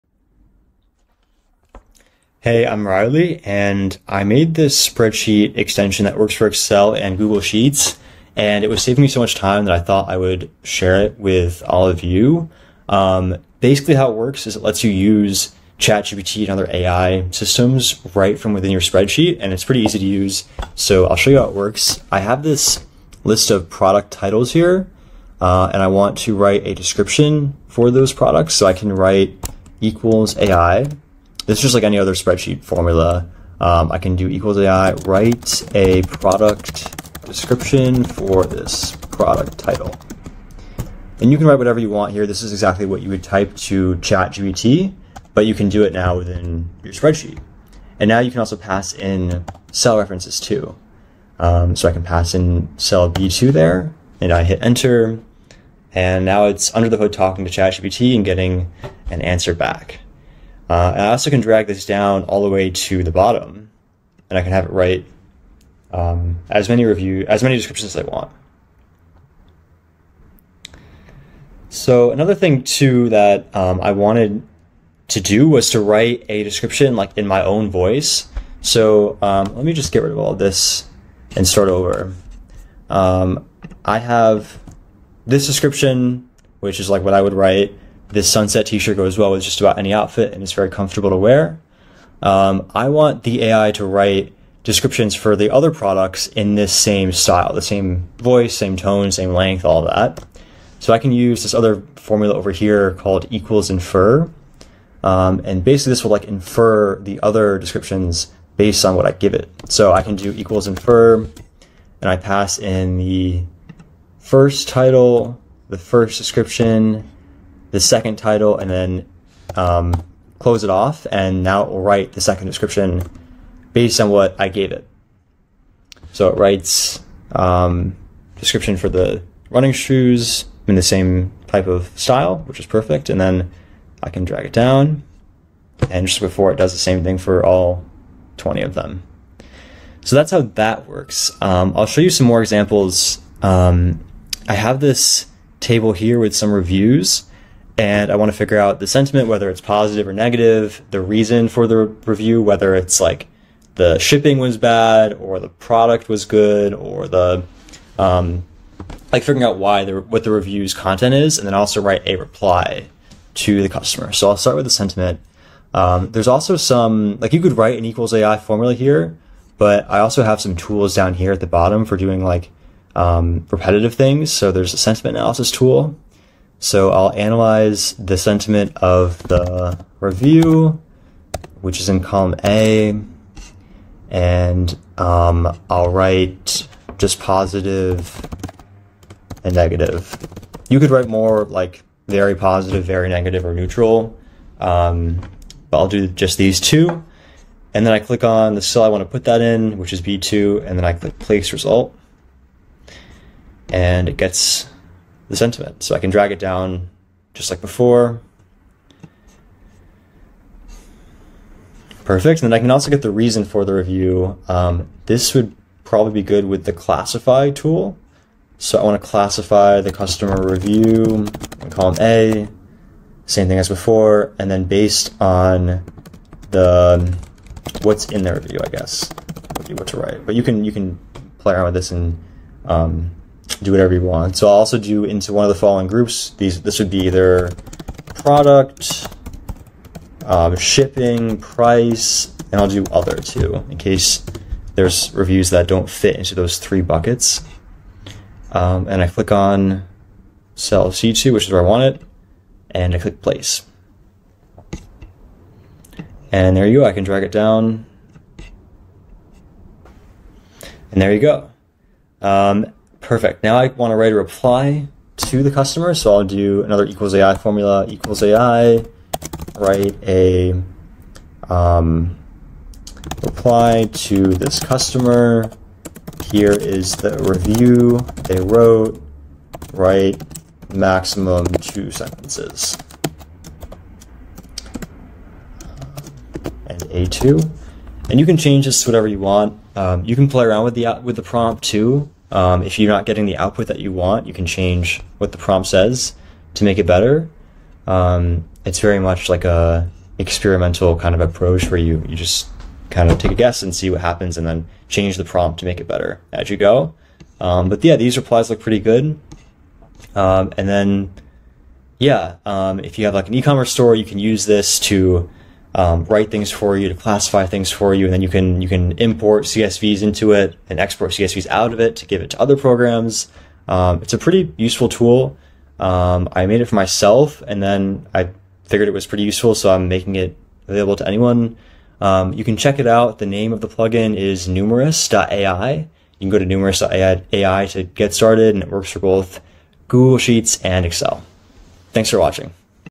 Using AI to write titles and descriptions for a Shopify store
Numerous.ai is a tool for Google Sheets and Excel to automate spreadsheet busywork.